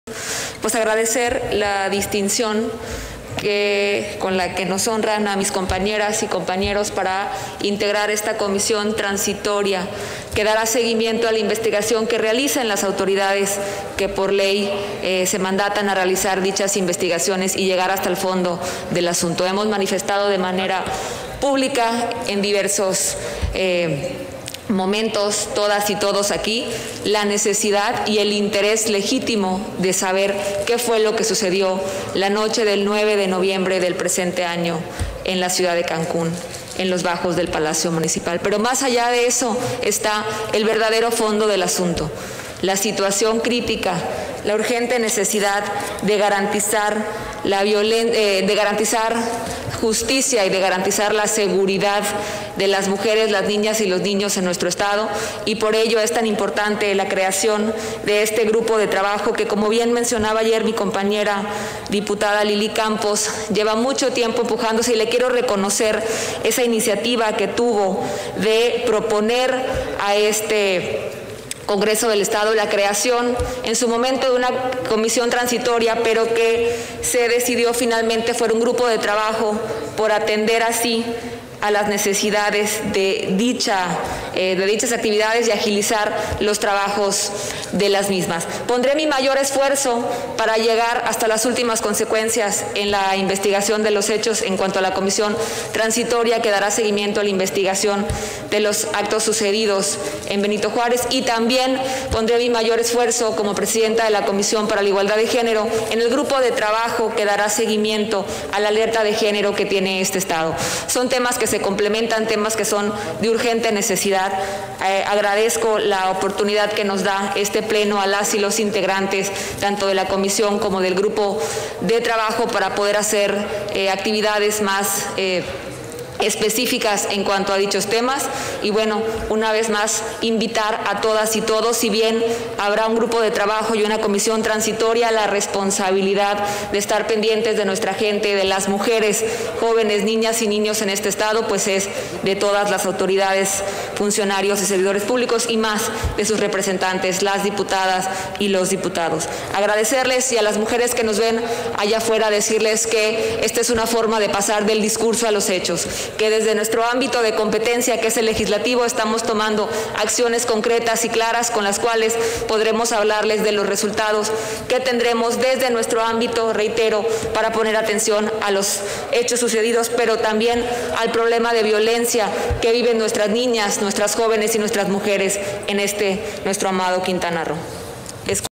Intervención de la Dip. Teresa Atenea Gómez Ricalde. PAN (11/Nov/2020)
Intervención de la Diputada Teresa Atenea Gómez Ricalde, del PAN, en relación al Acuerdo de la Junta de Gobierno y Coordinación Política de
la H. XVI Legislatura del Estado Libre y Soberano de Quintana Roo, por
el que se aprueba la creación del Grupo de Trabajo para la atención de
la Alerta de Violencia de Género contra las mujeres en Quintana Roo, del